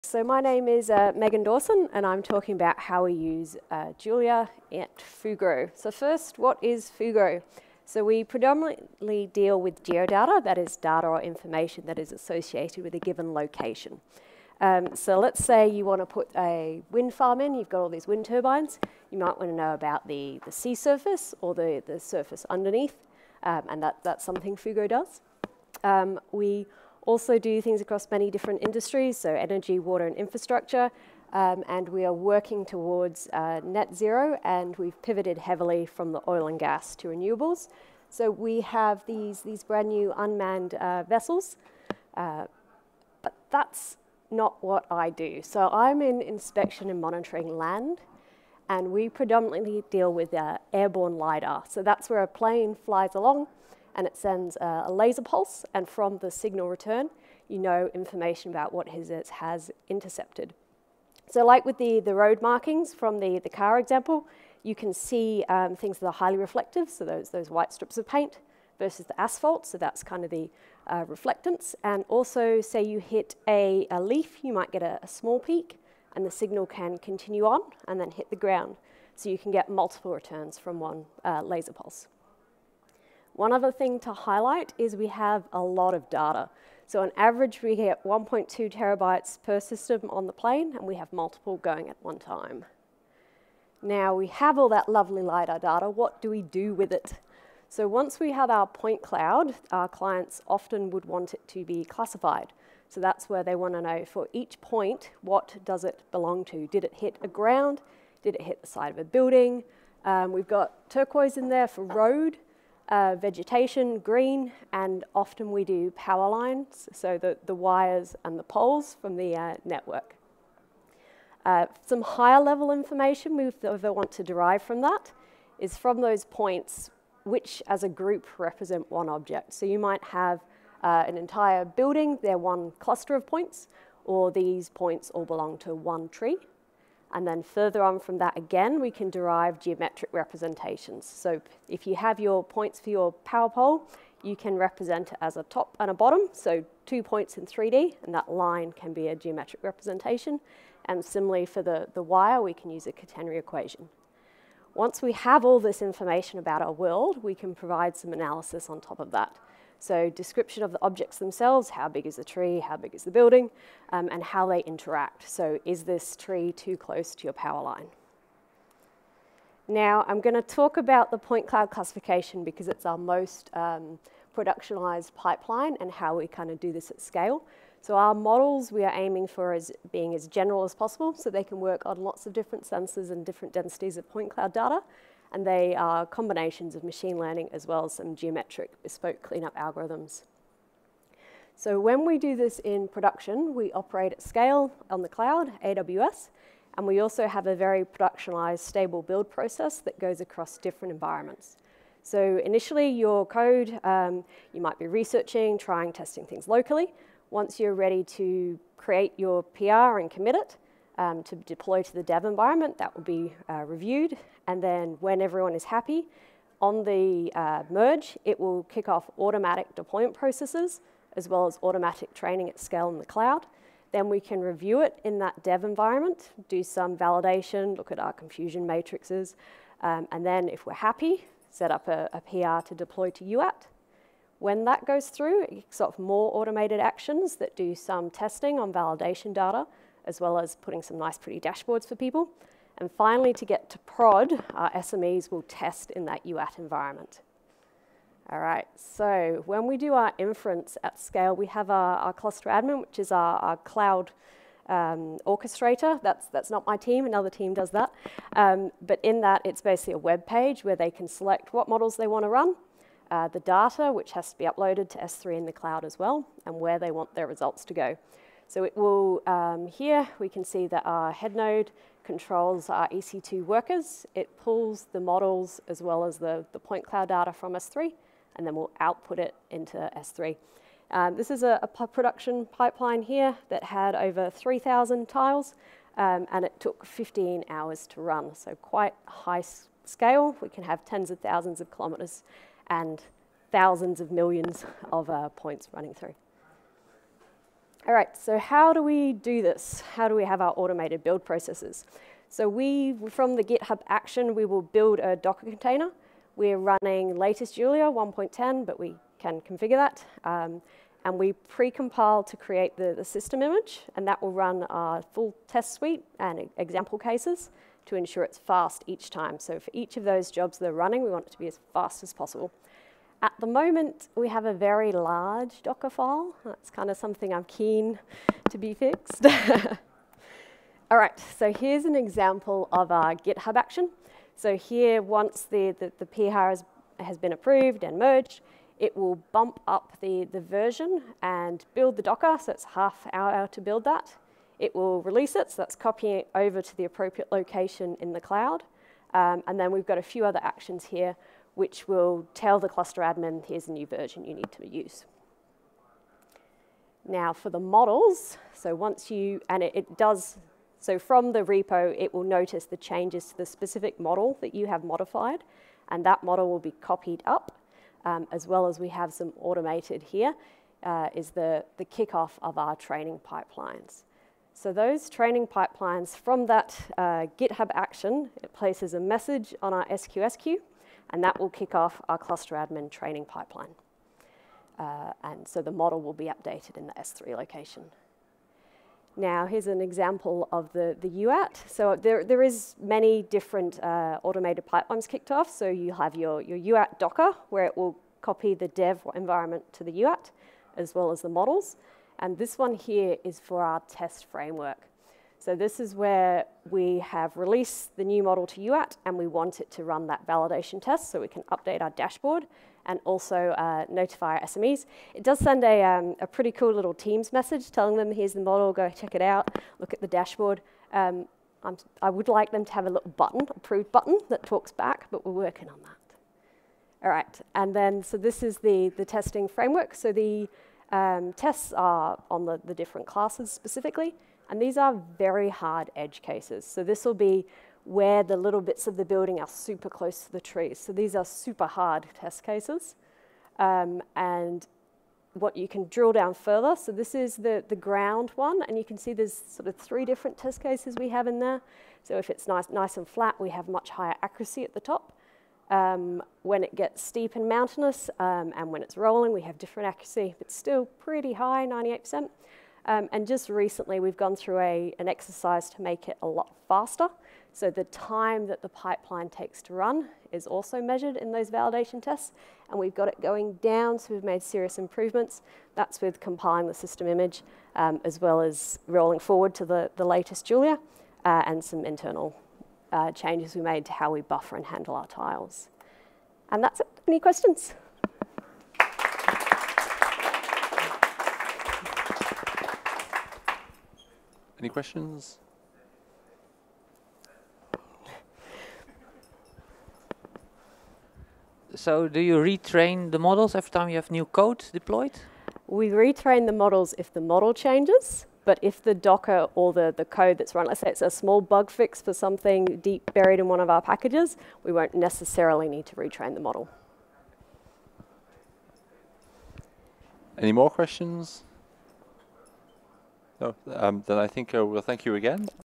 Julia at Fugro | Curran, Megan Dawson | JuliaCon 2024
Julia at Fugro by Ben Curran, Dr. Megan Dawson

Here at Fugro, we process large amounts of geospatial data in numerous HPC workflows to provide our clients with insights about real-world assets. Efficient processing enables faster delivery times for clients and reduced costs. We write our applications in Julia and have built systems using Github Actions that can seamlessly integrate these applications with cloud infrastructure hosted by Amazon Web Services (AWS).

In this talk, we will outline our process at Fugro to develop a production-ready HPC application with Julia. This will start with how we build these packages into binary system images and integrate them into docker images. We will then show how to use AWS and Github to deploy these docker images to a cloud environment, as well as some design considerations along the way to ensure you get the most out of your application. Throughout we will share our experiences and lessons learnt for anyone starting or considering using Julia in an industrial application.